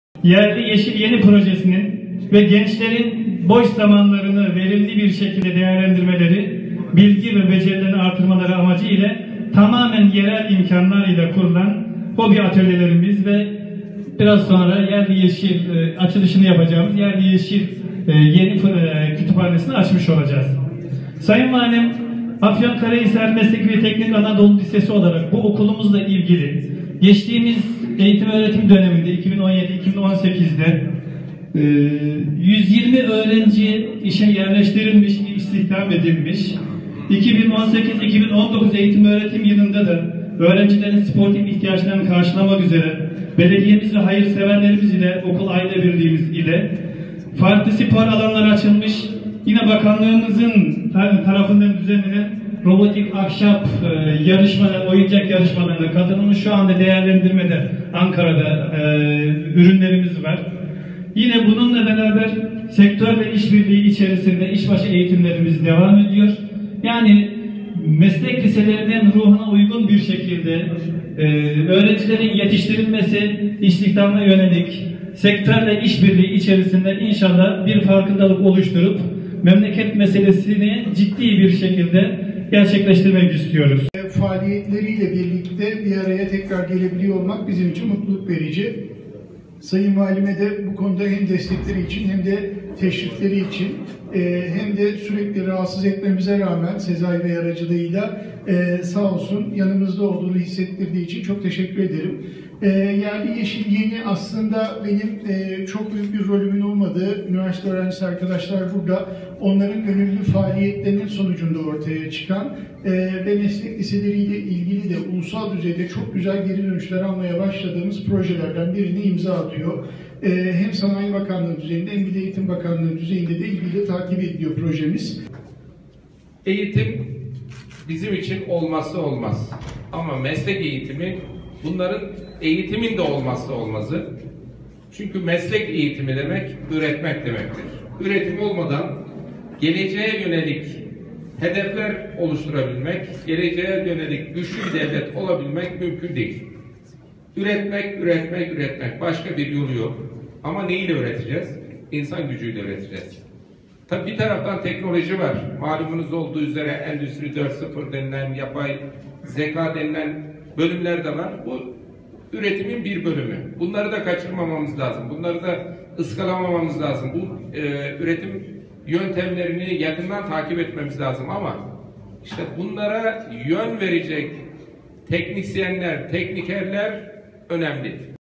Afyon menşeli Yerli Yeşil Yeni projesi Türkiye’ye örnek olacak
Afyonkarahisar Valisi Mustafa Tutulmaz, her halükarda nitelikli üreten insana ihtiyaç olduğunu bu mantıkla Afyonkarahisar’dan ortaya çıkan Yerli Yeşil Yeni Projesinin Türkiye’ye örnek olması noktasında iki bakanlığın dikkatini çektiğini söyledi.
Afyonkarahisar Mesleki ve Teknik Anadolu Lisesi’nde dün öğleden sonra ülkenin yerli üretim ve kapasitesine ilişkin farkındalık oluşturmak, mesleki e teknik eğitime farkındalığın arttırılması için oluşmuş Yerli Yeşil Yeni Projesi ve okulun Hobi Atölyesinin açılış töreni gerçekleştirildi.
“7 MESLEK LİSESİNE KATKI”
Törenin ilk konuşmasını İl Milli Eğitim Müdürü Metin Yalçın yaptı. Yalçın bu projeyle mesleki eğitime karşı farkındalık oluşturmak yerli, yeni, çevreci ve milli tasarımlarla ekonomiye destek olma amaçlandığını söyledi. AKÜ Öğretim Üyesi Doçent Dr. Murat Tiryakioğlu’nun katkısıyla gerçekleştirilen Yerli Yeşil Yeni Projesi ile gençlerin boş zamanlarını verimli şekilde değerlendirip, bilgi ve becerilerinin arttırılmasının ön görüldüğünü belirten Yalçın, “Tamamen yerli imkânlarla oluşturulan Hobi Atölyesi ve Yerli Yeşil Yeni Kütüphanesini hizmete açıyoruz. Okulumuzla ilgili geçen yıl ki öğrenim döneminde 120 öğrenci işe yerleştirilmiştir. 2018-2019 öğretim yılı itibariyle de öğrencilerin sportif ihtiyaçlarının karşılanması için farklı Belediye ve hayırseverlerin katkılarıyla spor alanları açılmıştır. İşbaşı eğitimlerimiz devam ediyor. İnşallah farkındalık oluşturarak memleket meselesini ciddi şekilde gerçekleştirmek istiyoruz. Açılacak bu kütüphanemiz 7 meslek lisesine katkı sağlayacaktır. Sayın Valimizin katkıları sağlanmıştır. İnşallah faydalı olacağını ümit ettiğimiz bu projenin hayırlı olmasını diliyorum.” dedi.
“PROJEYİ İKİ BAKANLIK TAKİP EDİYOR”
Yerli Yeşil Yeni Projesinin Program Kurucusu ve Koordinatörü AKÜ Öğretim Üyesi Doçent Dr. Murat Tiryakioğlu, 4 ay önce tanıtımını yaparak açılışını gerçekleştirdikleri projenin faaliyetlerinin kendilerini mutlu ettiğini kaydetti. Yerli Yeşil Yeni Projesinin gönüllü üniversite öğrencilerinin katkılarıyla ortaya konulduğunu söyleyen Tiryakioğlu, “Bu projeye çok güzel geri dönüşler alıyoruz. Projemiz Bilim Sanayi ve Milli Eğitim Bakanlıkları düzeyinde takip ediliyor. İtiraf etmek gerekiyor ki üniversite yerine Afyonkarahisar Mesleki ve Teknik Anadolu Lisesi’nde ve Okul Müdürü Osman Tezgiden hocamızla daha fazla mesai yapıyoruz. Gayet güzel sonuçlar almaya başladık. Bu nedenle başta Osman Tezgiden olmak üzere ev sahiplikleri nedeniyle tüm öğretmenlerimize çok teşekkür ediyorum. Kütüphaneler Sayın Valimizin himayesinde destek verilerek 7 okulumuz yararlanacak. İlk etapta ilkokulda kurulduktan sonra meslek liseleriyle beraber Afyon genelinde ve civar illerde de bu kütüphaneleri kurmaya devam edeceğiz.” diye konuştu.
“ESKİDEN ARA ELAMAN ŞİMDİ ARANAN ELAMAN”
Yerli Yeşil Yeni Kütüphanesine kitap hediye eden Afyonkarahisar Valisi Mustafa Tutulmaz açılış törenin son konuşmasını yaptı. Eğitimin olmazsa olmaz olduğu kadar mesleki eğitiminde eğitimin olmazsa olmazı olduğuna değinen Tutulmaz, “Meslek eğitimi demek öğretmek demektir. Öğretim olmadan geleceğe yönelik hedefler oluşturabilmek mümkün değildir. Üretmekten başka bir yol yok. Ama ne ile üretmek? Üretimi insan gücü ile yapacağız. Üretimin teknoloji boyutunu kaçırmamız lazımdır. Ama bunlara yön verecek teknisyenler, teknikerler önemlidir. Eskiden ara eleman denilen elemanlar şu anda aranan elemanlardır.” şeklinde konuştu.
“İHTİYAÇ İŞİNİ İYİ YAPAN İNSANLAR”
Çok sayıda mühendisin olduğunu ancak fabrikaları ve atölyeleri işletecek ara elemanlara ihtiyacın arttığını belirten Tutulmaz şöyle konuştu:
“Bizim işini iyi yapan insanlara ihtiyacımız var. İşini iyi yapanlar ister kamu da, ister özel sektörde isterse kendi işinde olsun ekonomik açıdan da sıkıntı çekmiyorlar. Her halükarda nitelikli üreten insana ihtiyacımız var. İşte Yerli Yeşil Yeni Projesi bu mantıktan ortaya çıkmış bir projedir. Üniversitemizle Milli Eğitim arasında bu proje ile çok güzel bir sinerji yakalandı. Toplumun ihtiyaçları yan yana getirildi. Bu gerçekten örnek bir uygulamadır. Afyon’dan çıkan bu proje Türkiye’ye örnek olması yönüyle iki bakanlığın dikkatini çekmiştir. Bu uygulamanın Türkiye’nin değişik bölgelerine ülkenin genel bir politikası olabilir mi çalışmaları devam etmektedir. Valilik olarak imkanlarımız ölçüsünde projenin başından sonunu kadar yanında olduk. Projenin hayata geçmesinin önemli olduğunu düşünüyorum. Afyonkarahisar olarak sanayileşmenin ve üretimin hızla ilerlediği bir şehirde aranan elemana çok ihtiyacımız var. Projeye emeği geçen herkese teşekkür ediyorum.” Burcu AYDIN’ın Haberi